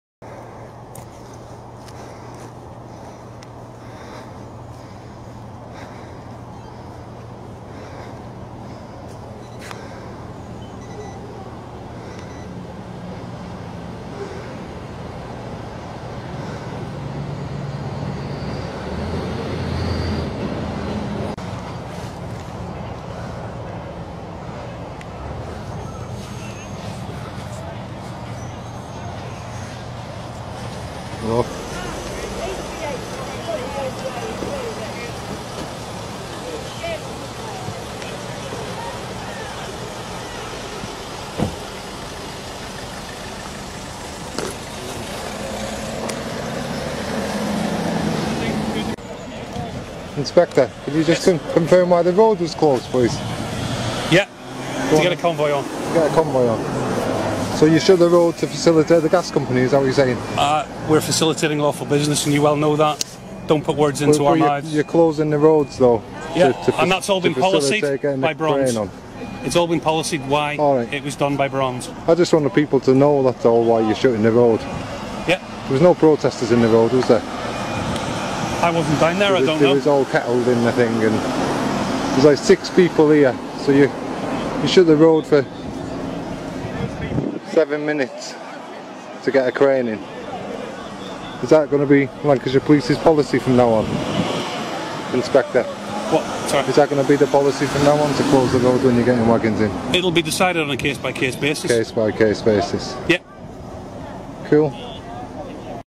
PNR: Lancashire Police Protecting Cuadrilla Convoy 08082017
Inspector confirming road closure due to "getting convoy in" lol i couldnt believe it, i almost started laughing at him when he said it :) i felt like saying did you not get the memo titled blame everything on them? ~Jon O'Connor.

MAJORITY of road closures on Preston New Road are to help trucks get the turn on the road that requires both lanes... NOT because any Protectors are about - they conveniently 'kettle' any of us in advance ie: holding vans and line of police to keep us in place and out of the way. ~Tina Louise Rothery.

Preston New Road, Little Plumpton, Lancashire,
Frack Free Lancashire, Frack Off, Cuadrilla,